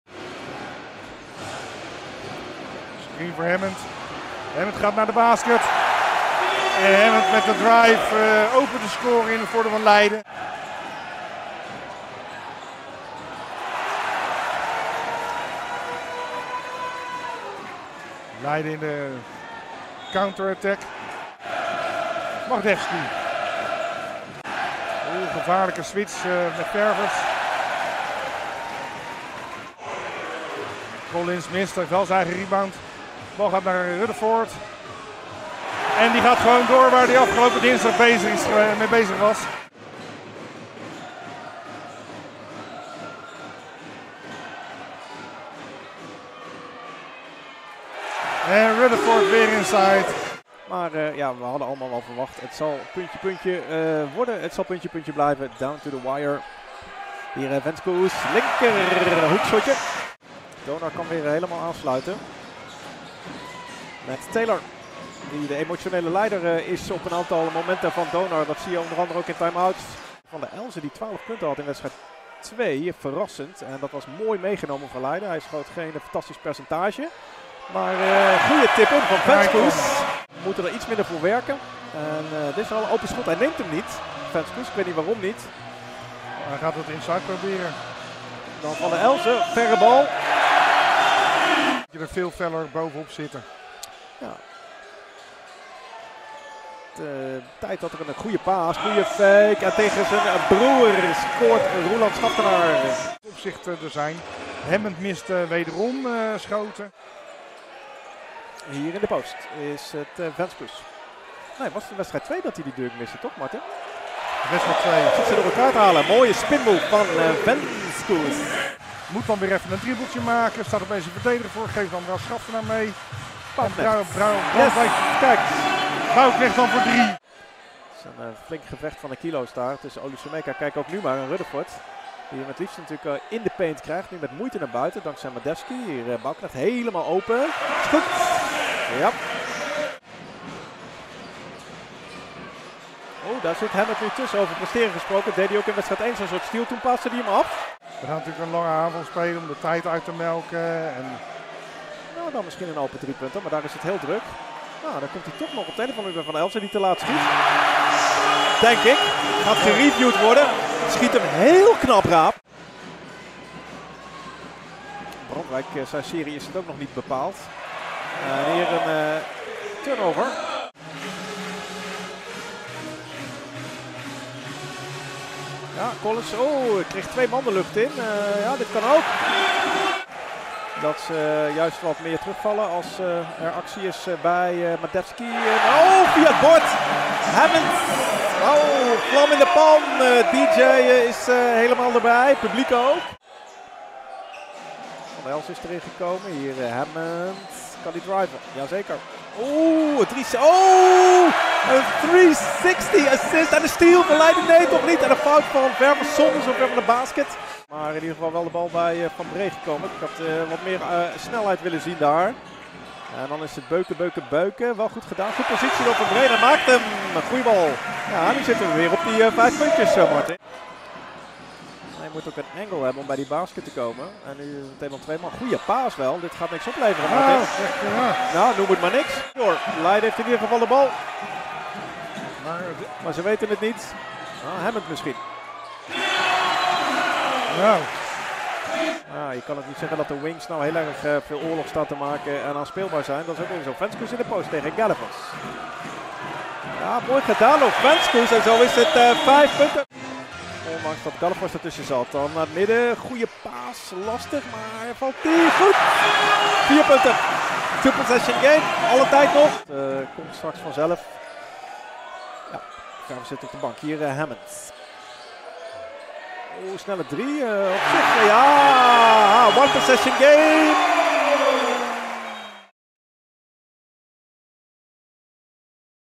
Korte samenvatting Zorg en Zekerheid Leiden - Donar Groningen, finale Game 3 (25 mei 2023)
De korte samenvatting van deze wedstrijd in de BNXT League, met de belangrijkste acties van Leidse zijde. Met dank aan BNXT.tv
Het is wedstrijd 3 in de Finale van de Nederlandse playoffs, een best-of-5 serie. De stand is daarin 1-1.

ZZ Leiden (blue-green):
0 David Collins
2 Maarten Bouwknecht
3 Deion Hammond
4 Nezar Kanbier
8 Liam van der Schalie
9 Stan van den Elzen
10 Roeland Schaftenaar
11 Duko Bos
13 Luuk van Bree
17 Lucas Kruithof
21 Erikas Venskus
42 Thomas Rutherford
Douglas Spradley Hoofdcoach
Jan Stalman Assistent-coach
Martin Houwaard Assistent-coach

Donar Groningen (white):
0  Stephaun Branch
2  Vernon Taylor
5  Leon Williams
7  Sander Hollanders
9  Andrej Magdevski
17  Peter Olisemeka
18  Viktor Gaddefors
19  Willem Brandwijk
27  Mark Roorda
30  Olaf Schaftenaar
32  Kjeld Zuidema
61  Clay Mounce